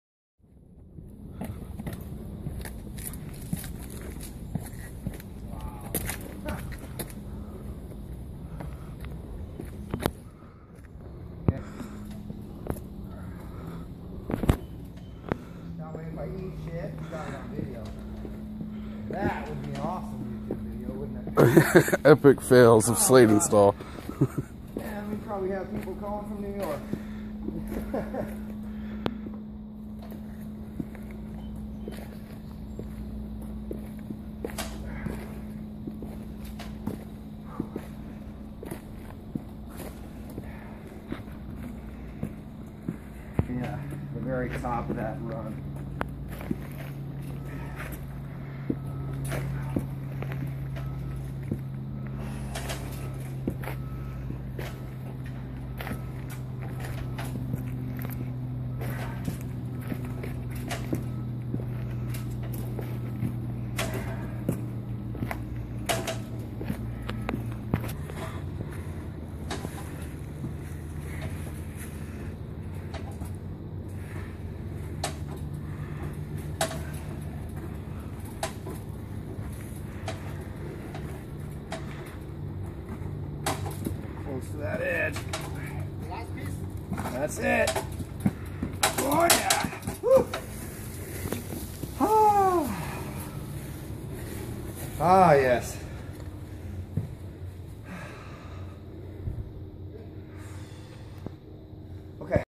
EPIC POOL TABLE DELIVERY!
This is one of the most challenging pool table installations ever. Almost vertical stairway down to Lake Washington. There are over 100 primitive stairs to get down to the final location